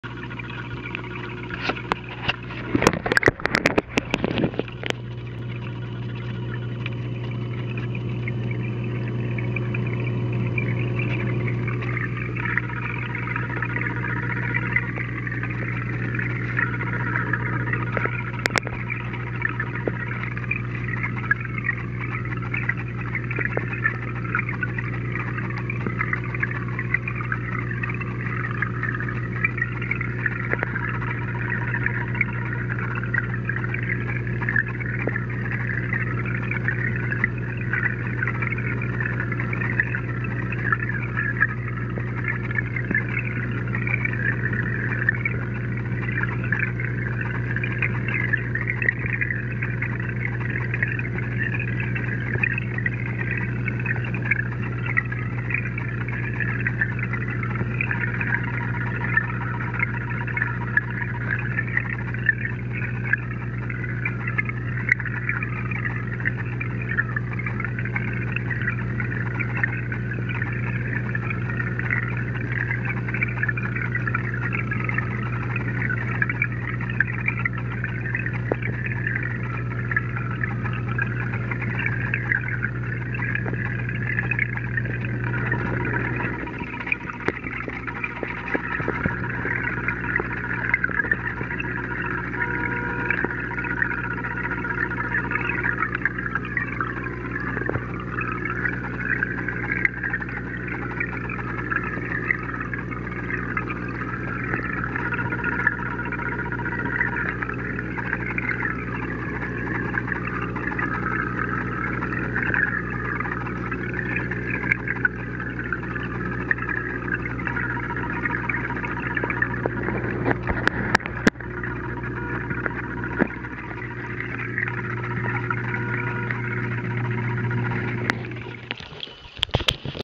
Underwater in my saltwater fish tank using Panasonic DMC TS1 in motion jpeg, mjpeg
Testing out my Panasonic DMC TS1 underwater in my 50 gallon salt water fish tank.  This is in Motion JPEG mode, so this is a .mov file.
(velvet damsel, clownfish, true percula, Ocellaris Clownfish, six line wrasse, anemone )